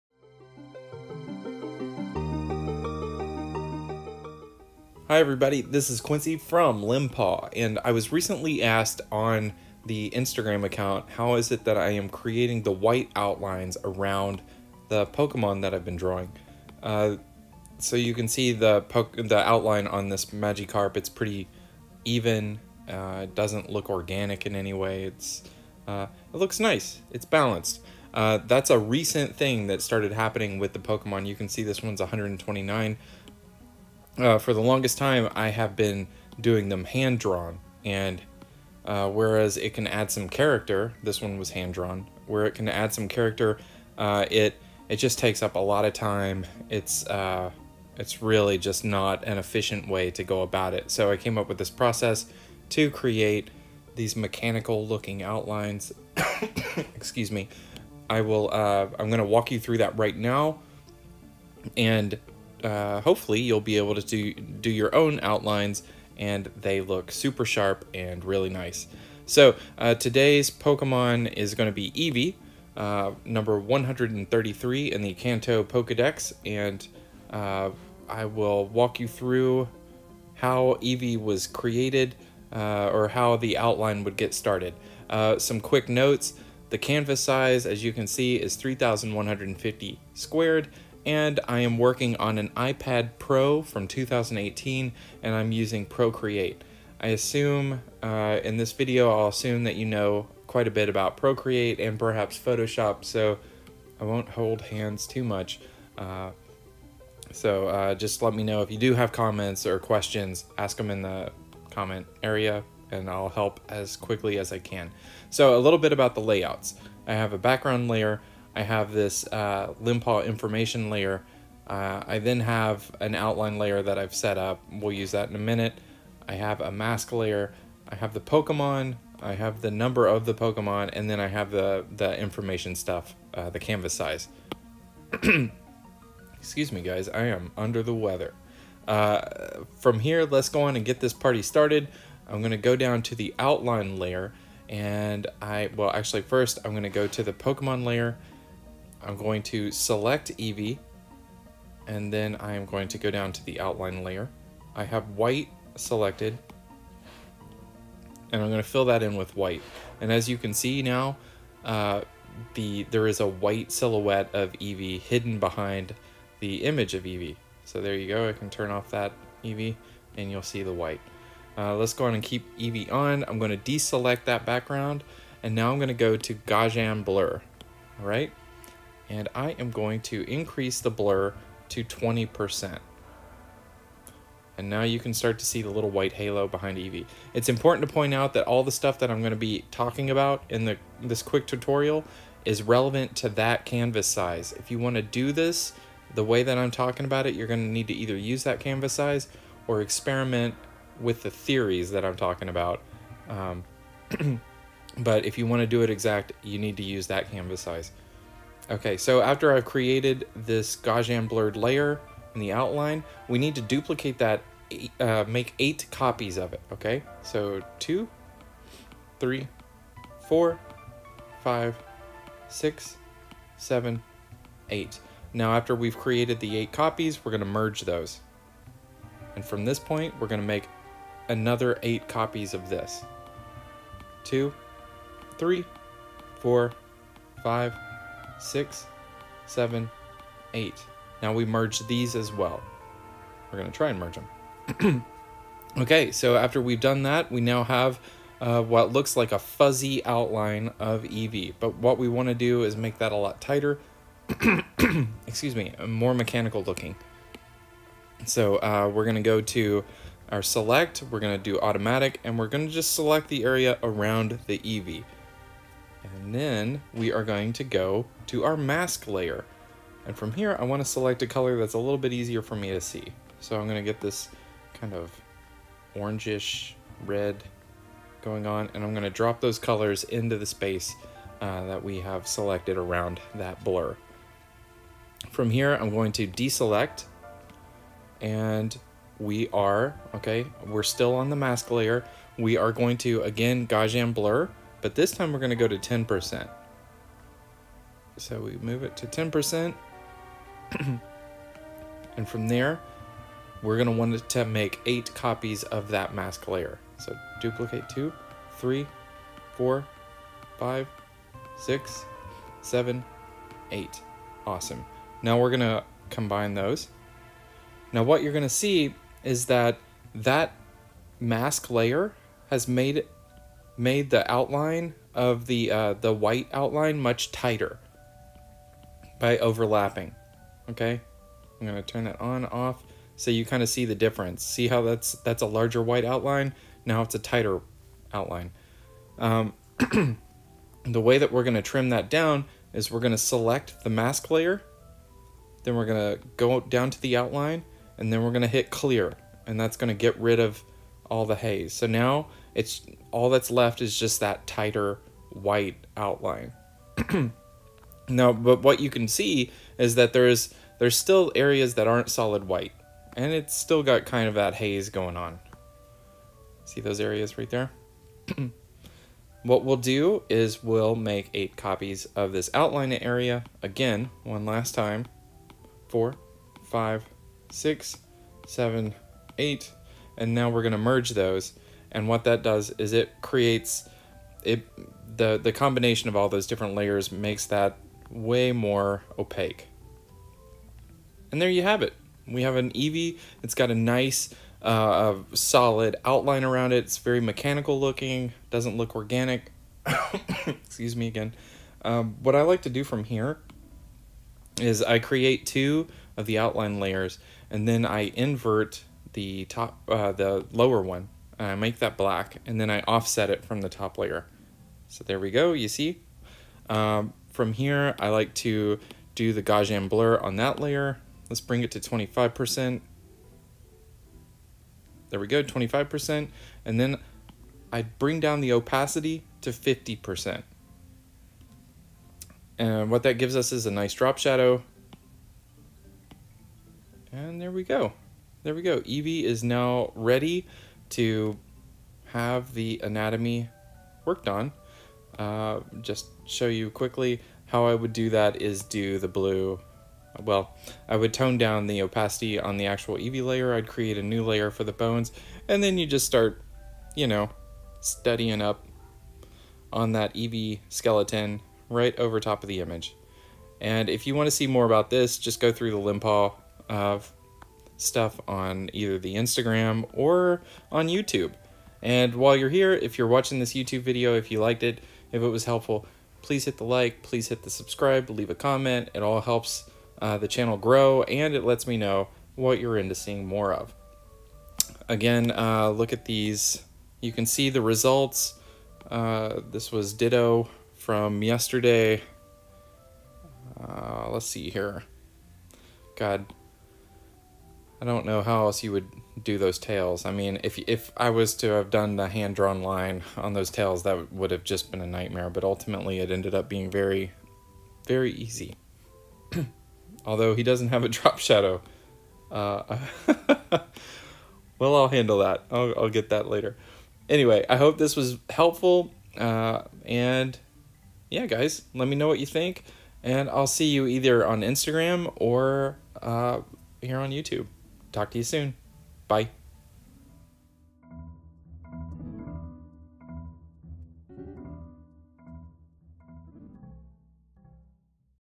How To Make Outlines In Procreate - iPad Pro 2018
Learn how to make outlines in Procreate, while using the iPad Pro (2018). - This is the fast and easy technique I use to outline my huge library of Pokemon fan art!

Please join us on Instagram (@Limbpaw), Share the video, and let us know what you'd like to see in the comments.